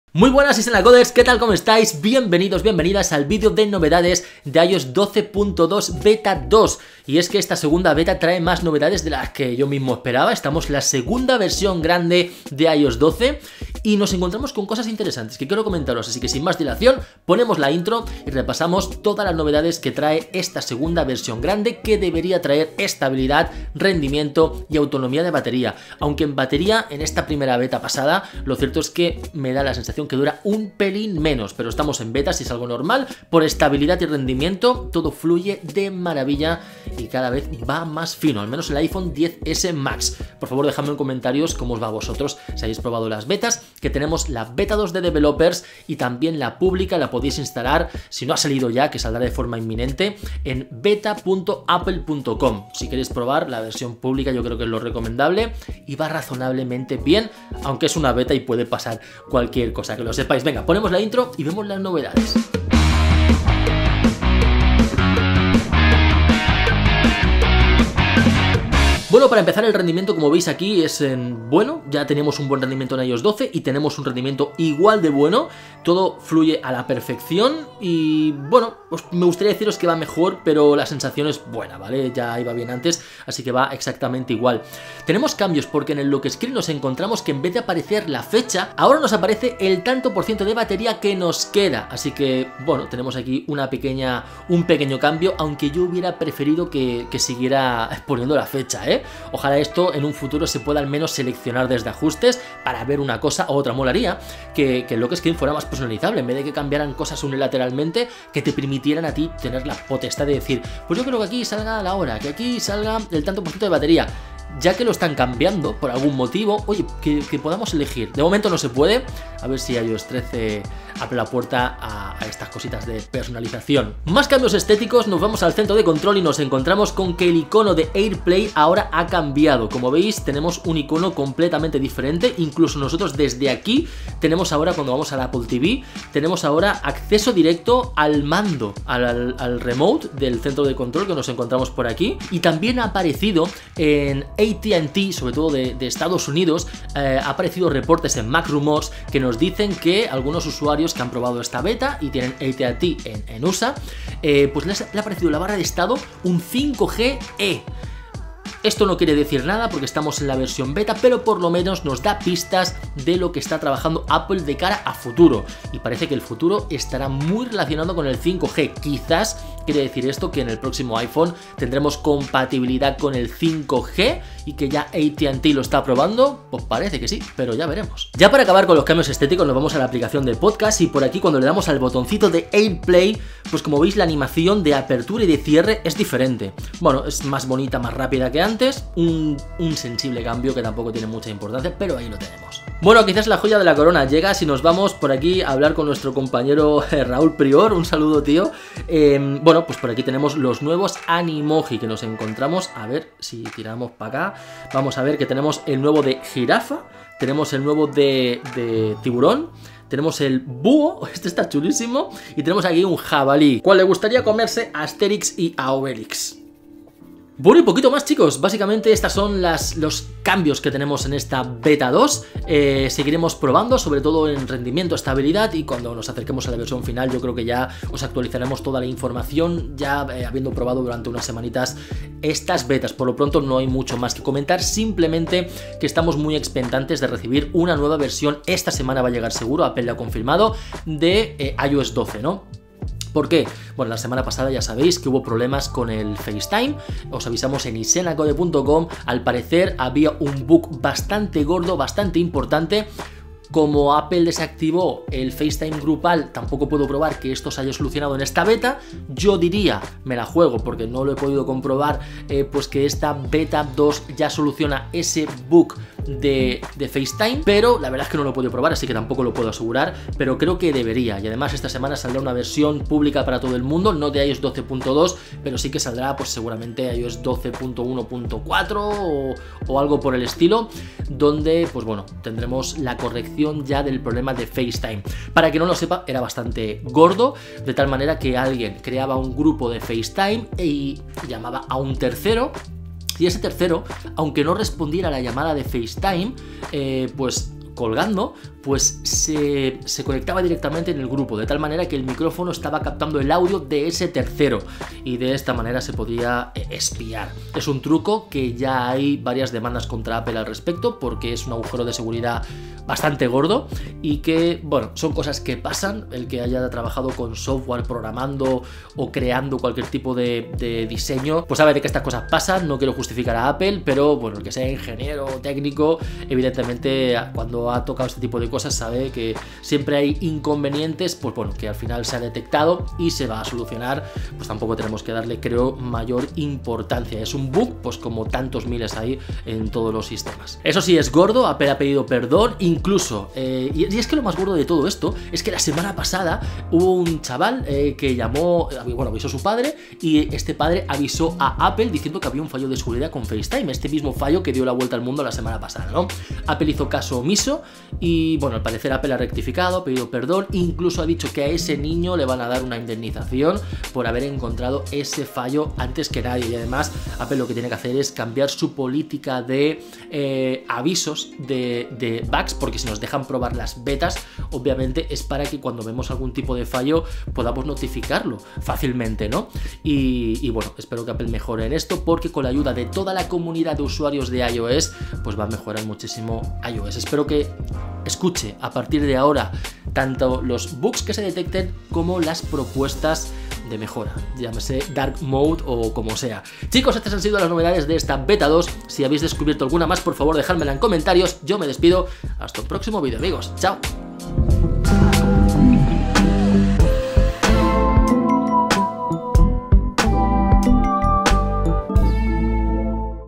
iOS 12.2 beta 2 ¡Novedades!
EN EL VÍDEO DE HOY: repasamos todas las novedades de iOS 12.2 beta 2.

👍 Recupera tu Apple ID o salta la pantalla de bloqueo, si olvidaste la contraseña de tu iPhone, con iMyFone LockWiper